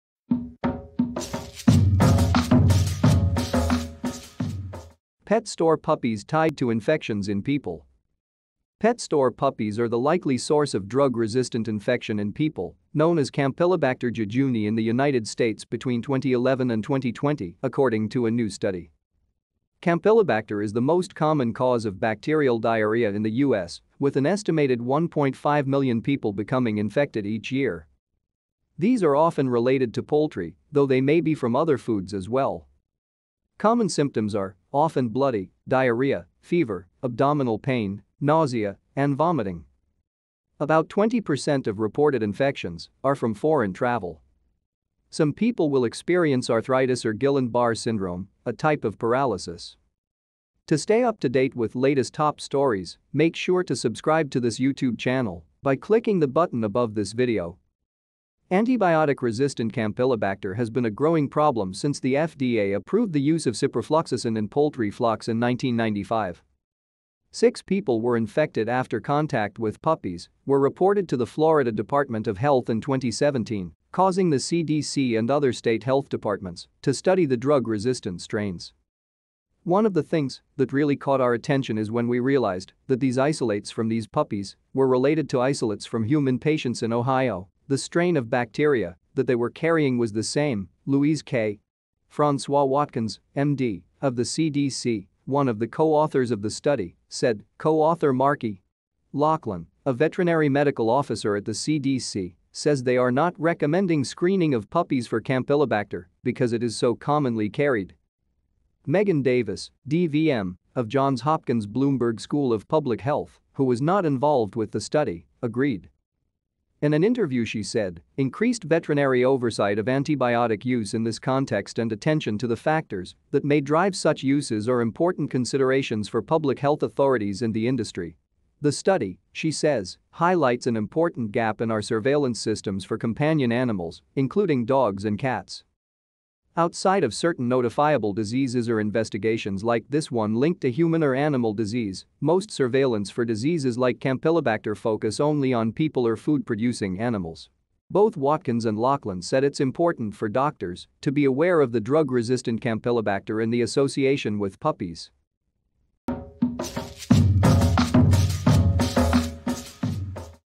Pet Store Puppies Tied to Infections in People
Pet store puppies are the likely source of drug-resistant infection in people known as Campylobacter jejuni in the United States between 2011 and 2020, according to a new study.
Campylobacter is the most common cause of bacterial diarrhea in the U.S., with an estimated 1.5 million people becoming infected each year.
These are often related to poultry, though they may be from other foods as well.
Common symptoms are (often bloody) diarrhea, fever, abdominal pain, nausea, and vomiting.
About 20% of reported infections are from foreign travel.
Some people will experience arthritis or Guillain -Barre syndrome, a type of paralysis.
Antibiotic-resistant Campylobacter has been a growing problem since the FDA approved the use of Ciprofloxacin in poultry flocks in 1995.
Six people were infected after contact with puppies were reported to the Florida Department of Health in 2017, causing the CDC and other state health departments to study the drug-resistant strains..